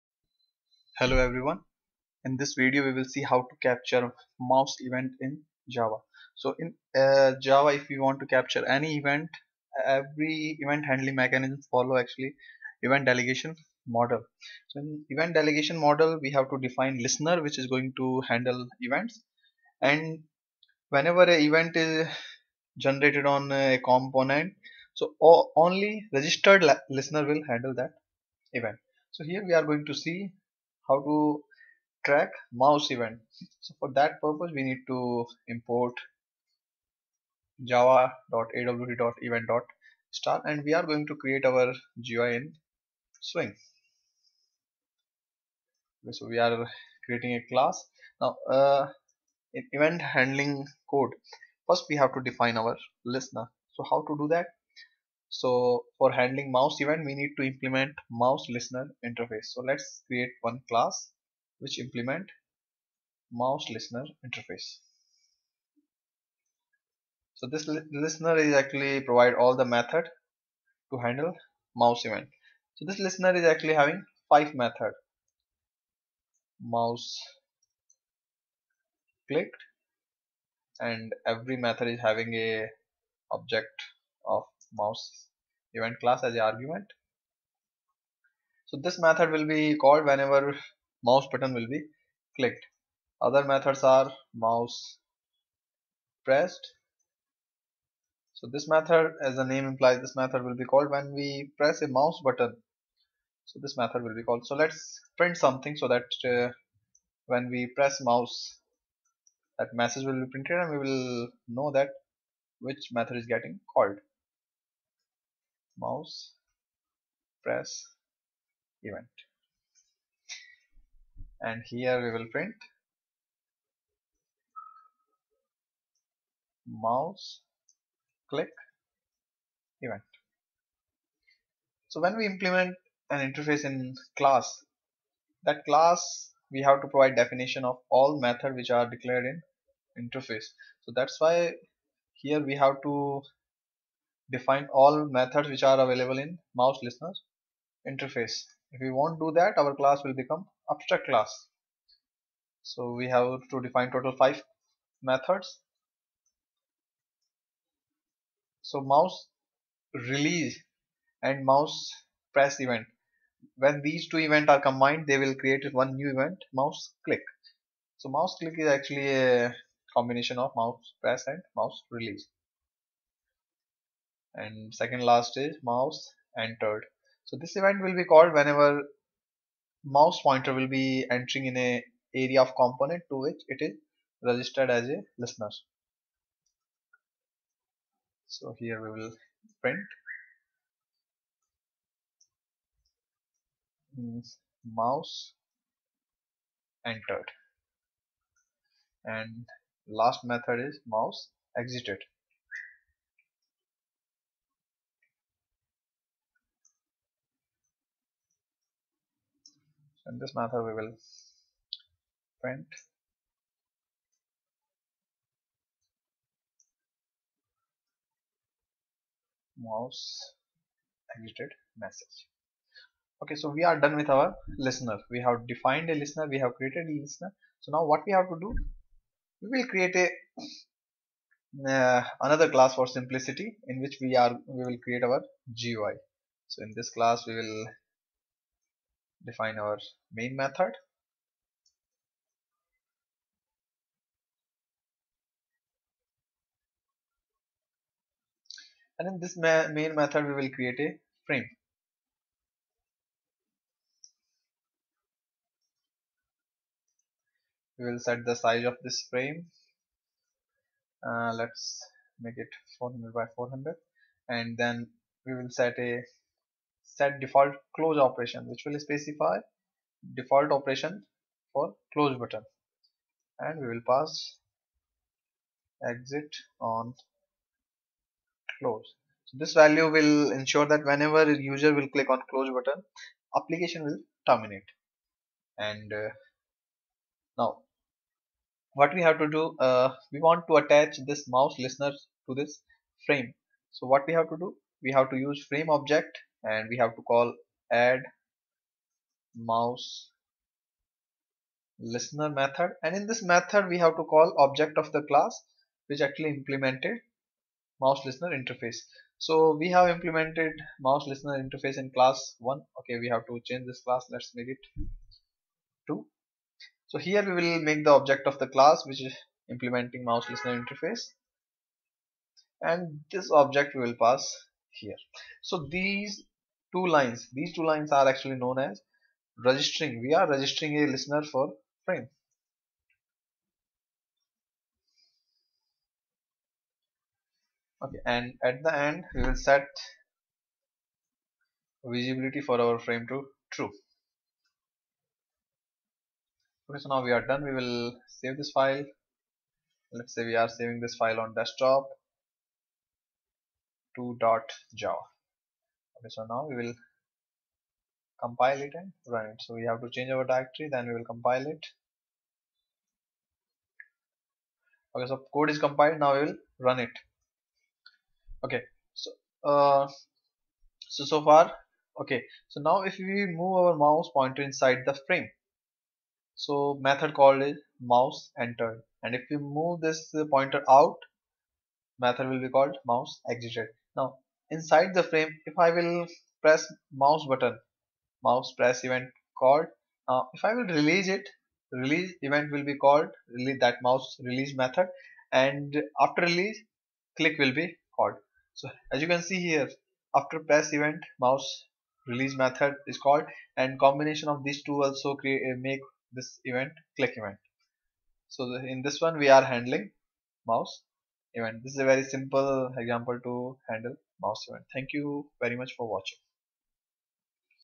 Event Handling : Mouse Event Handling In Java
MouseListener interface is used for defining listener for handling mouse event.
This interface has five methods
mouseClicked
mousePressed
mouseReleased
mouseEntered
mouseExited